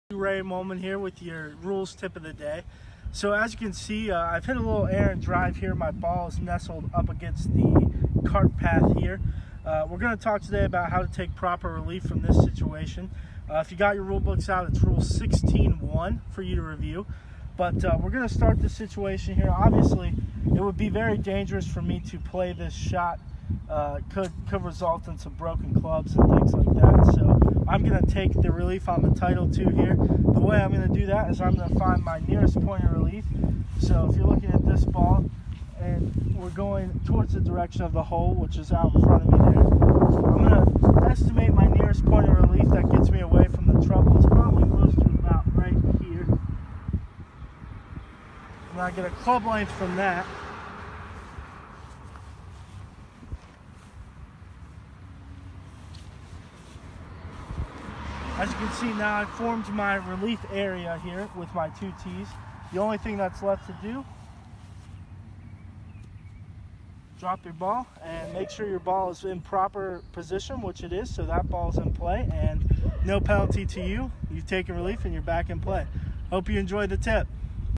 Golf House Rules Tip of the Day - Relief from Cart Path (and other Immovable Obstructions)
Ray shows us how to take relief from a cart path, which also covers relief from things like animal holes, immovable obstructions and temporary water.